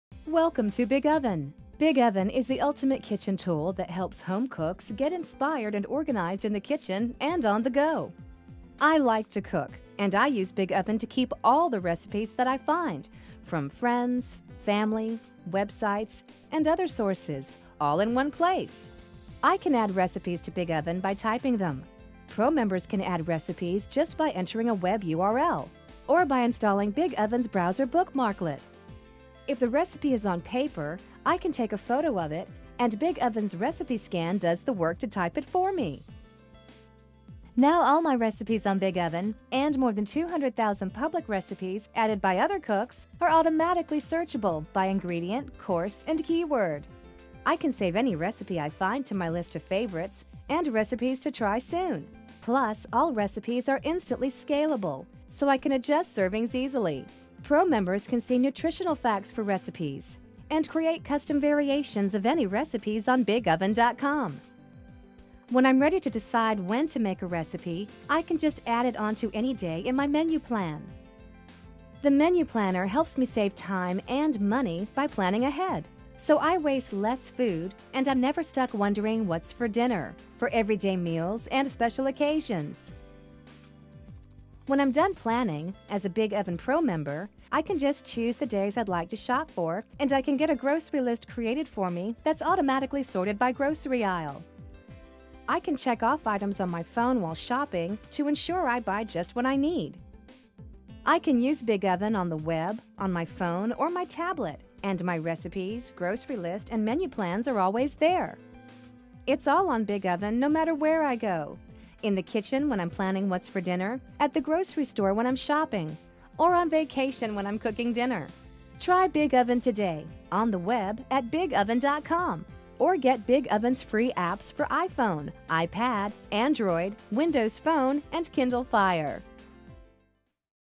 BigOven Organizes Your Recipes
BigOven is an easy recipe organizer for iPhone, iPad, Android, Kindle Fire, the Web and Windows Phone.

Keep your recipes here, and bring them anywhere!  Create menu plans, grocery lists and more.  Search 200,000+ recipes, including your own, at the grocery store.